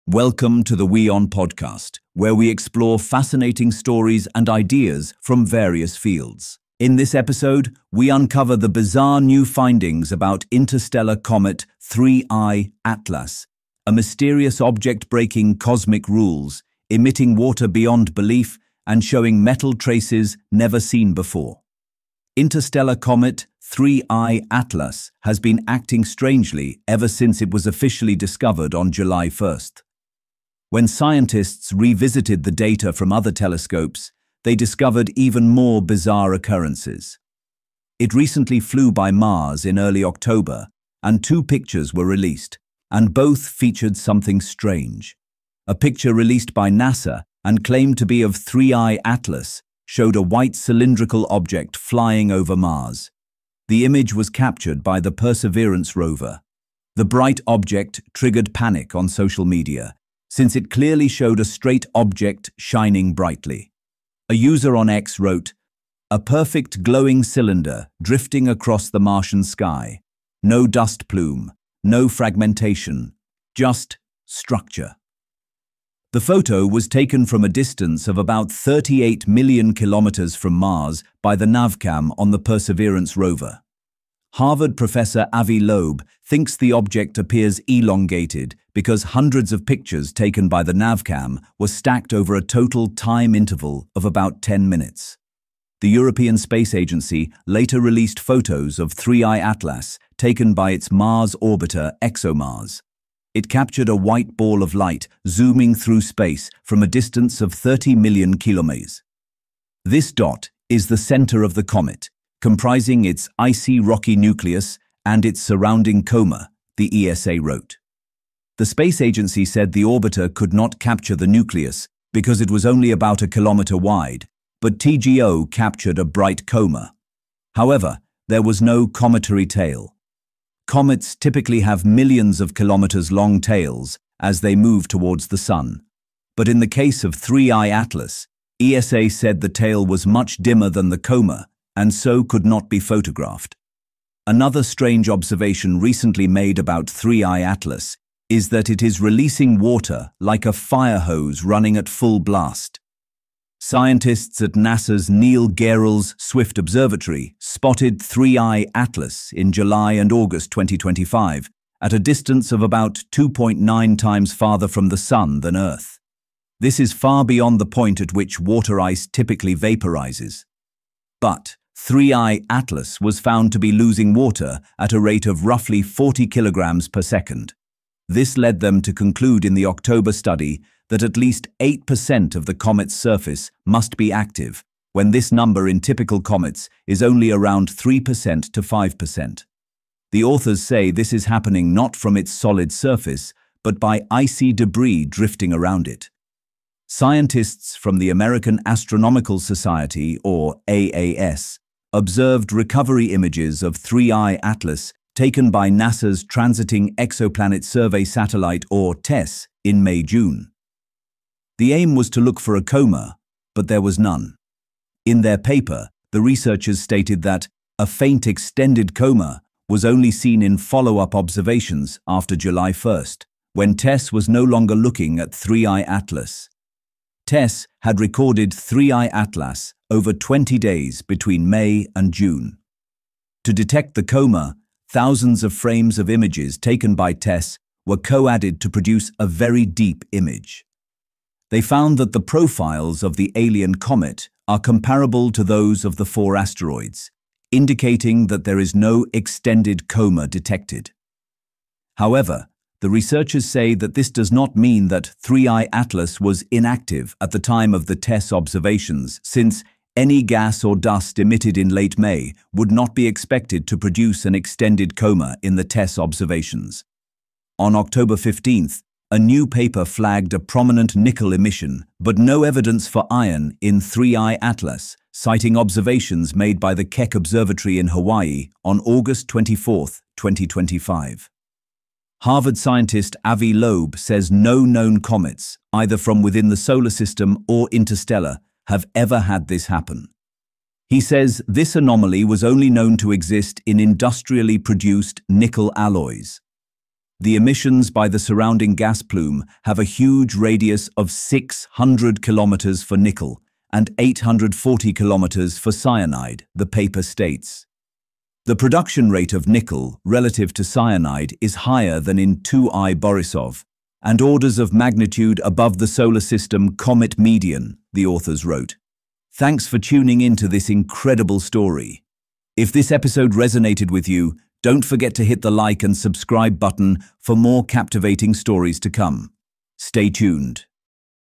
3I/ATLAS: Alien Comet Defies Science with Strange Metal Emissions | WION Podcast
In this episode, we uncover new, baffling findings about interstellar comet 3I/ATLAS, a mysterious object breaking the known laws of space science. From releasing water far beyond the Sun’s heat range to showing nickel emissions without iron, 3I/ATLAS is rewriting what scientists know about comets. Captured by NASA, ESA, and Keck Observatory, this object has left experts stunned — with some even suggesting it could be an artificial or alien relic. Join us as we decode the strange signals, glowing metals, and cosmic questions surrounding this outlier of the universe.